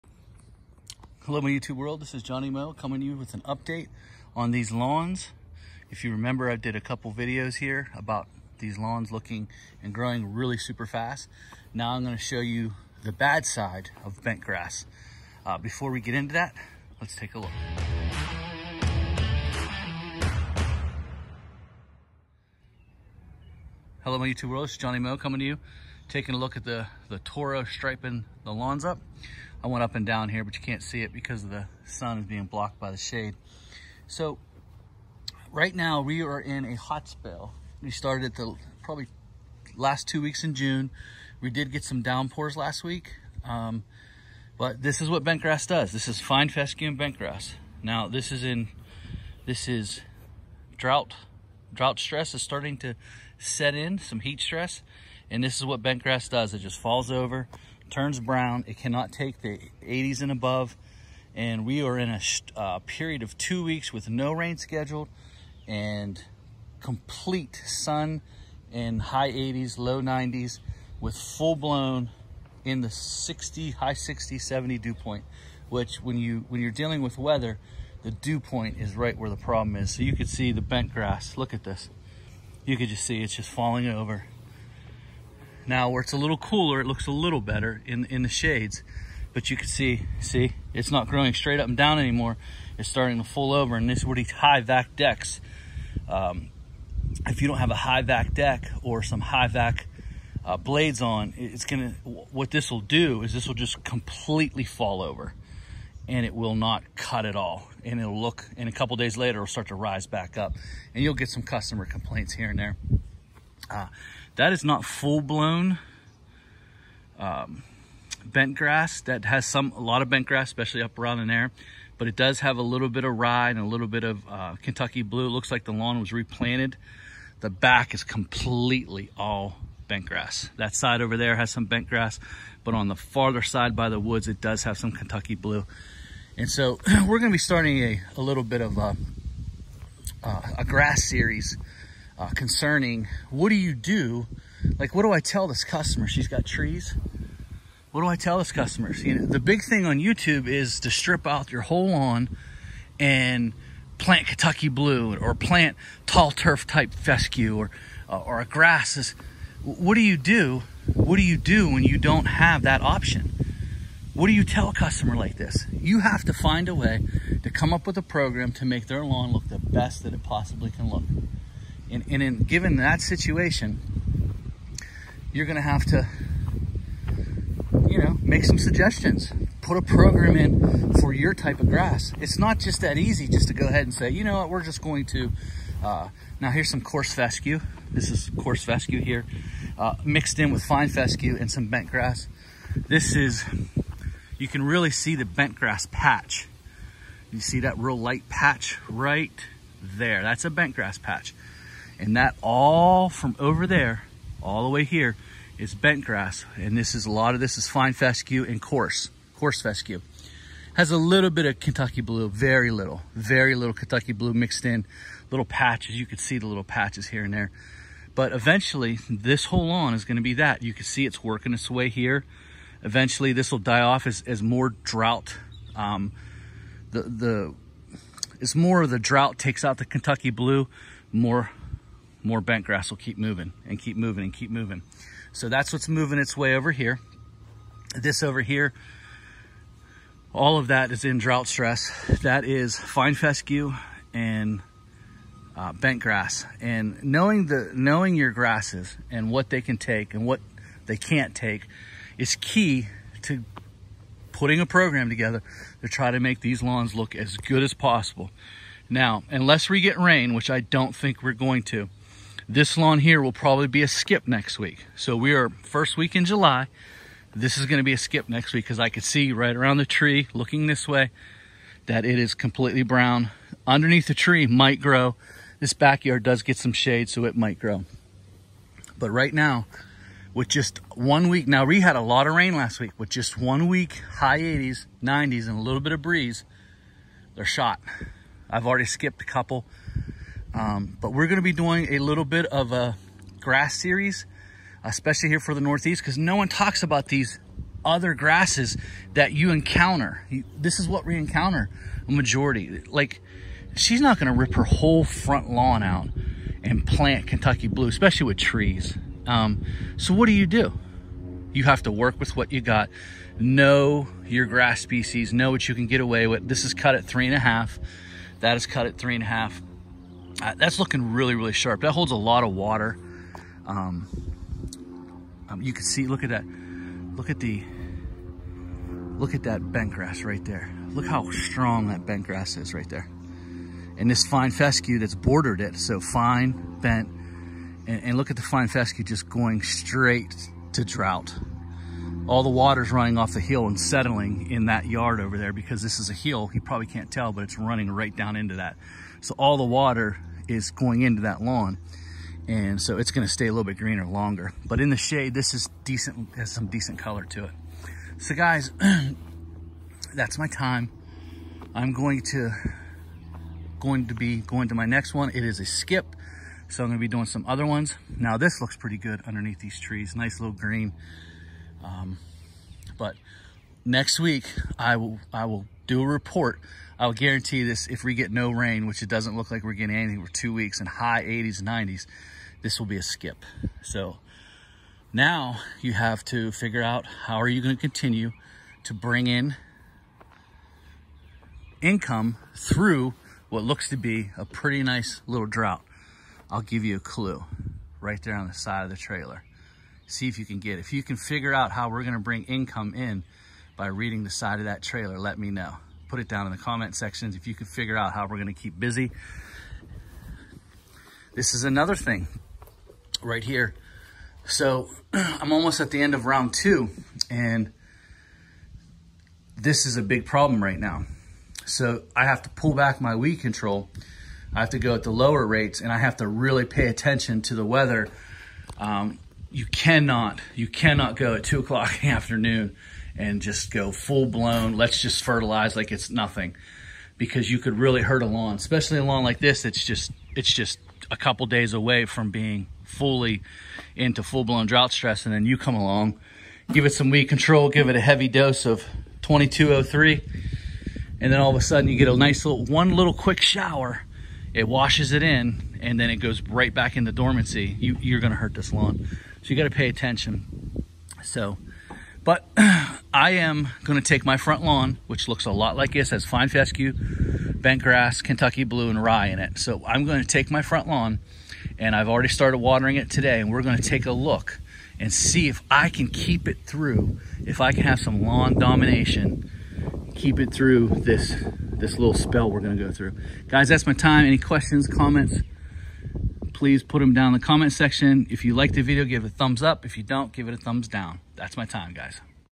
What do you tell your customer
Today we talk about grass species and drought stress.